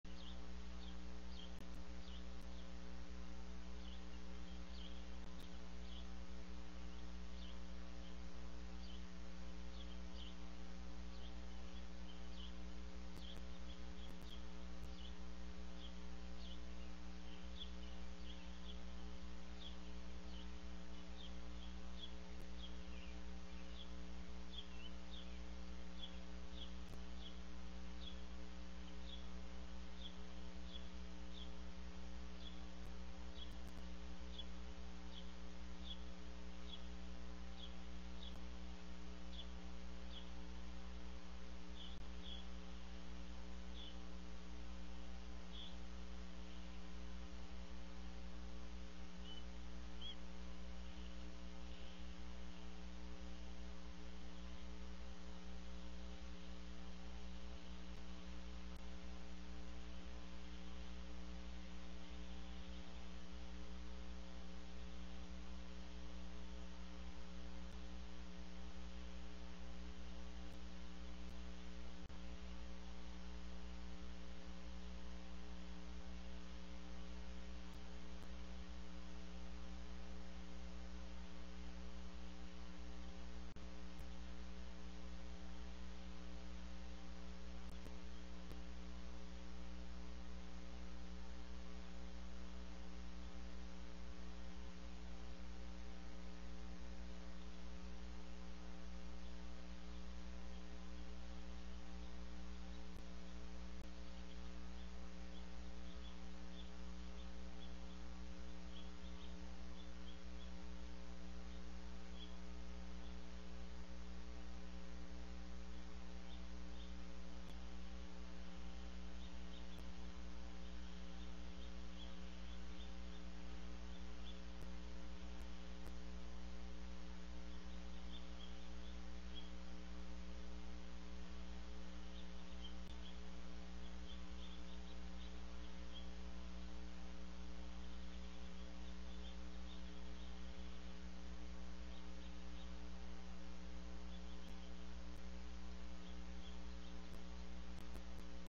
IL Trio Eagles: Valor 1 Stick Playing 3-13-19
While brooding, Valor 1 was playing & moving sticks. So cute when he tries to twirl them.